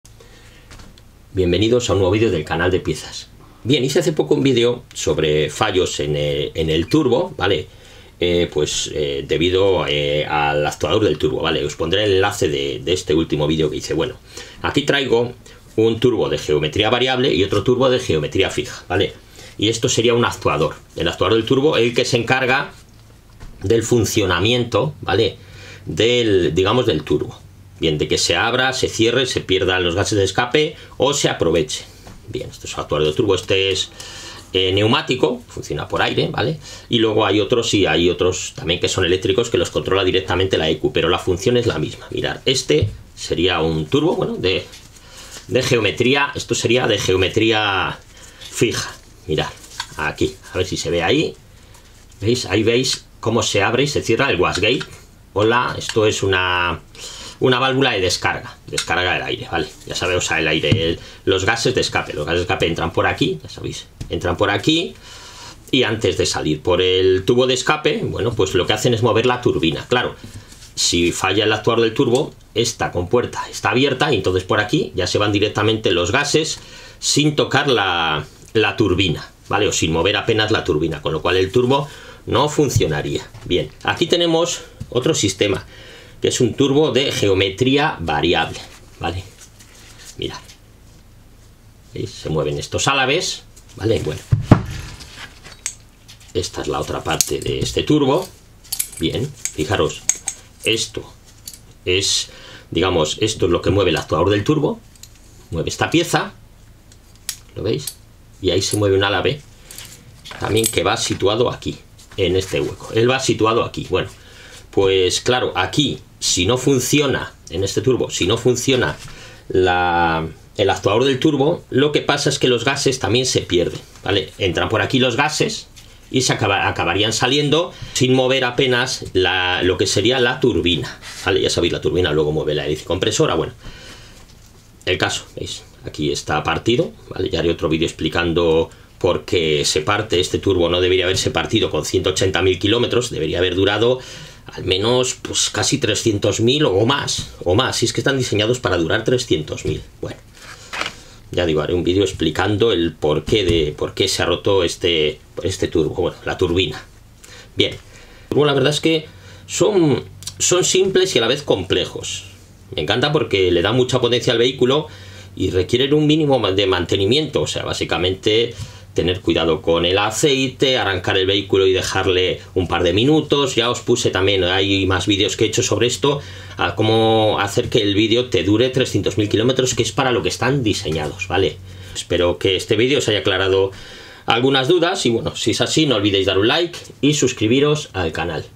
FALLOS de TURBO de geometría FIJA y VARIABLE; wastegate, pulmón/actuador del turbo, turbina, álaves
Fallos de los turbos de geometría fija y variable